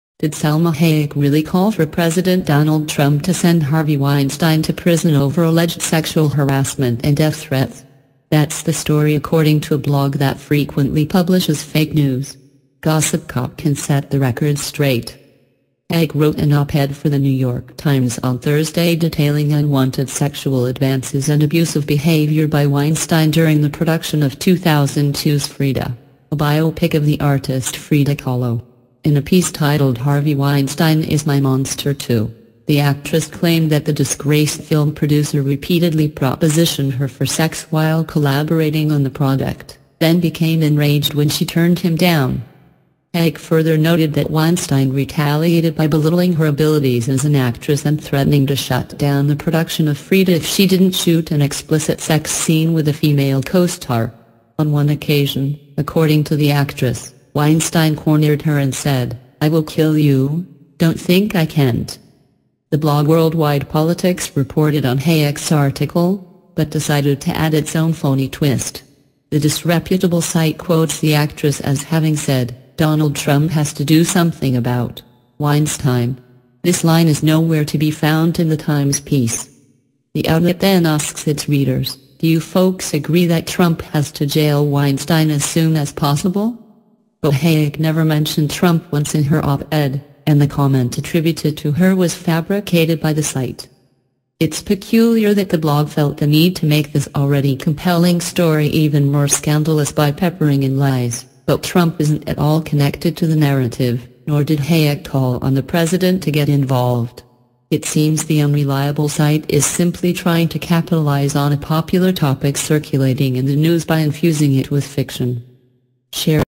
Salma Hayek Calling For Donald Trump To Send Harvey Weinstein To Prison
Wellcome to our channel. I am showing News in this video Salma Hayek Calling For Donald Trump To Send Harvey Weinstein To Prison

But any kind of mistake i am say sorry.

Did Salma Hayek really call for President Donald Trump to send Harvey Weinstein to prison over alleged sexual harassment and death threats? That’s the story according to a blog that frequently publishes fake news. Gossip Cop can set the record straight.

Hayek wrote an op-ed for the New York Times on Thursday detailing unwanted sexual advances and abusive behavior by Weinstein during the production of 2002’s Frida, a biopic of the artist Frida Kahlo. In a piece titled “Harvey Weinstein Is My Monster Too,” the actress claimed that the disgraced film producer repeatedly propositioned her for sex while collaborating on the project, then became enraged when she turned him down.